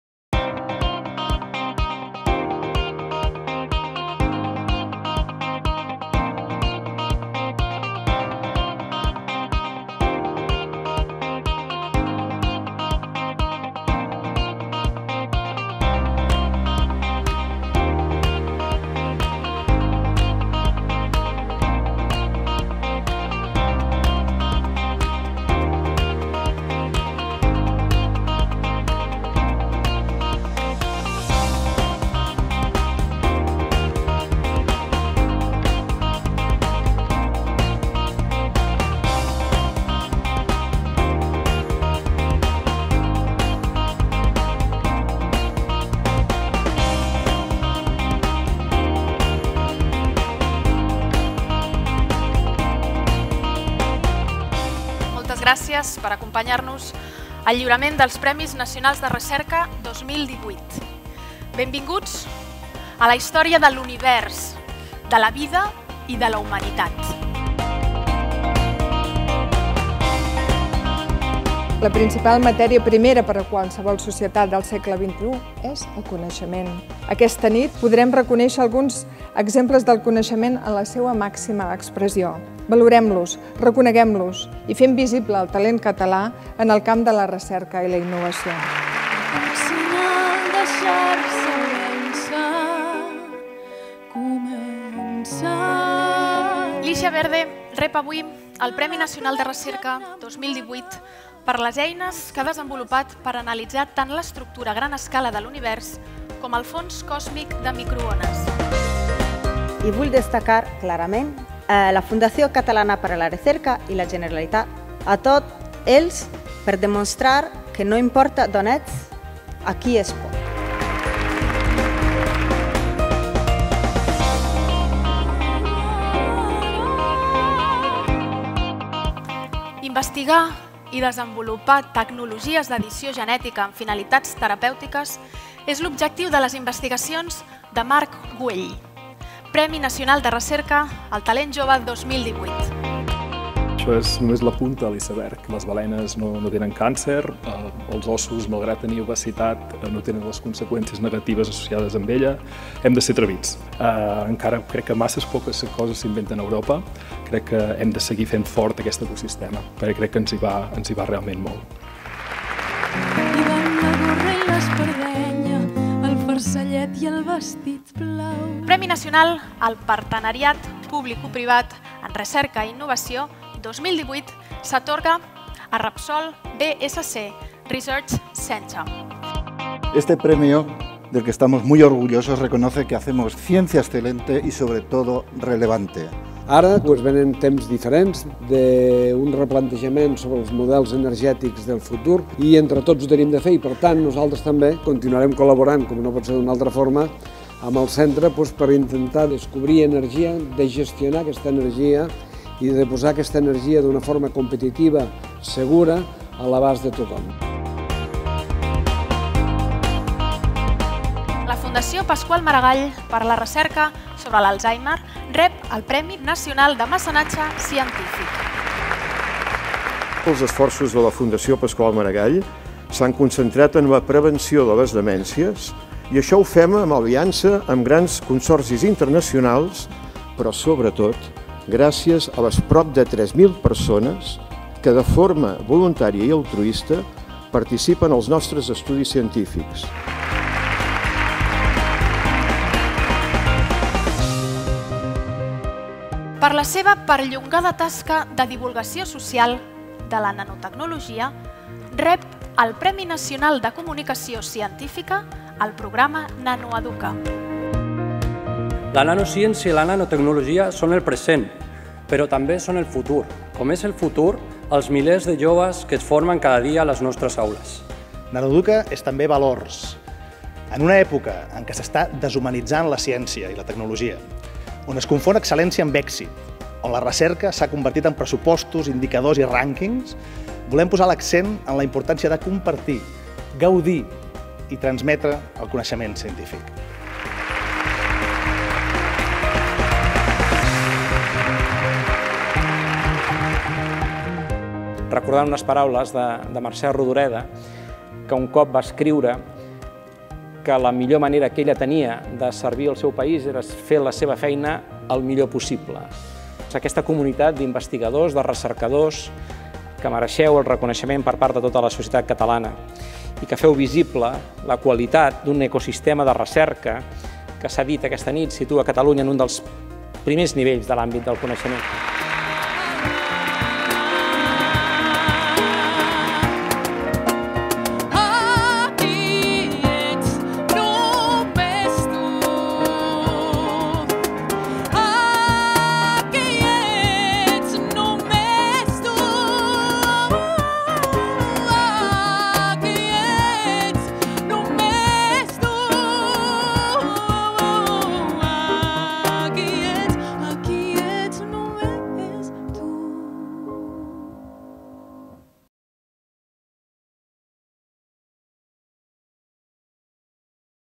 Vídeo resum acte lliurament 29PNR
Els Premis Nacionals de Recerca, convocats pel Govern de Catalunya i la Fundació Catalana per a la Recerca i la Innovació, fomenten el reconeixement social de la ciència i l'activitat dels investigadors, mecenes, empresaris i comunicadors.

L’acte de lliurament dels Premis Nacionals de Recerca 2018, presidit pel M. Hble. Sr. Quim Torra, president de la Generalitat de Catalunya, es va celebrar el dimarts 3 de desembre, al Teatre Nacional de Catalunya (plaça de les Arts, 1) de Barcelona, a les 18.00 hores. La gala va ser conduïda per l’actriu i cantant Elena Gadel.

Els guardonats de la 29a edició van ser: Licia Verde, professora d’investigació ICREA a l’Institut de Ciències del Cosmos de la Universitat de Barcelona (ICCUB), Premi Nacional de Recerca; Marc Güell, investigador principal del Grup de Recerca en Biologia Sintètica Translacional del Departament de Ciències Experimentals i de la Salut de la Universitat Pompeu Fabra (UPF), Premi Nacional de Recerca al Talent Jove; la Fundació Pasqual Maragall per a la Recerca sobre l’Alzheimer, Premi Nacional de Mecenatge Científic; el Repsol-BSC Research Center (RBRC), Premi Nacional al Partenariat Publicoprivat en R+I; i programa NanoEduca (ICN2-UB-UAB-CESIRE), Premi Nacional de Comunicació Científica.